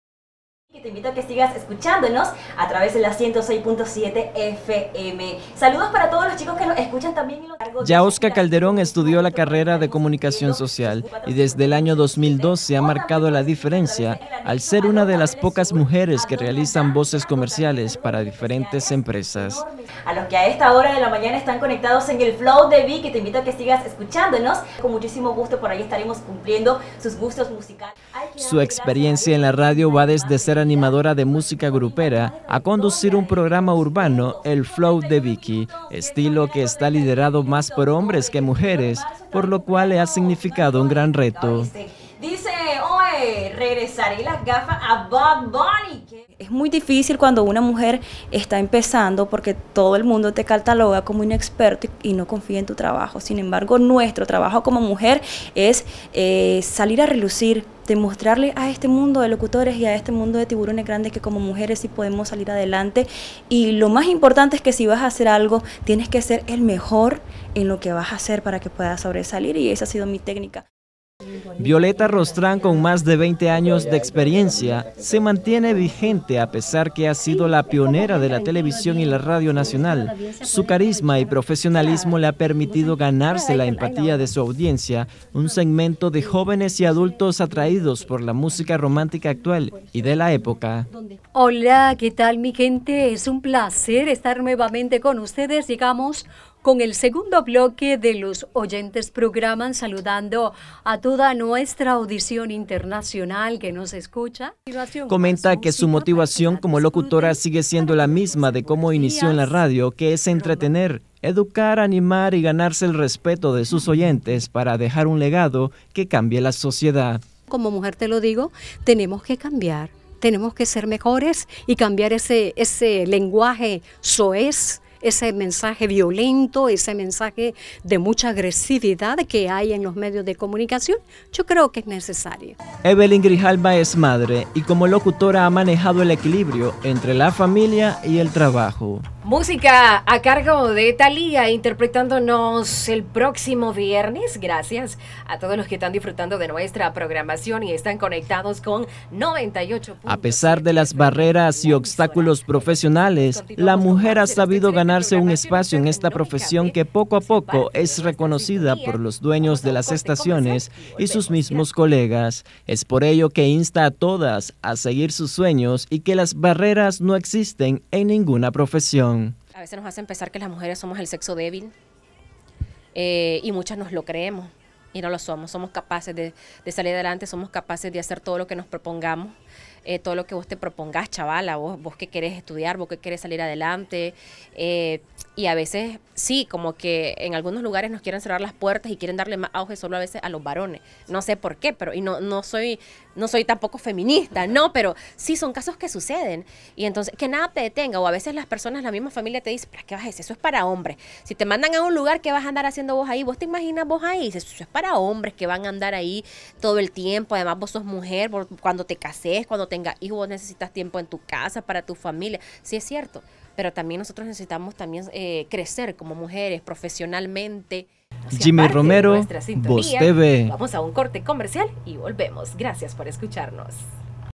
Locutoras se apoderan de las radios en Nicaragua
Tres grandes mujeres que incursionaron en la radio han logrado ganarse el cariño, aprecio y admiración de los oyentes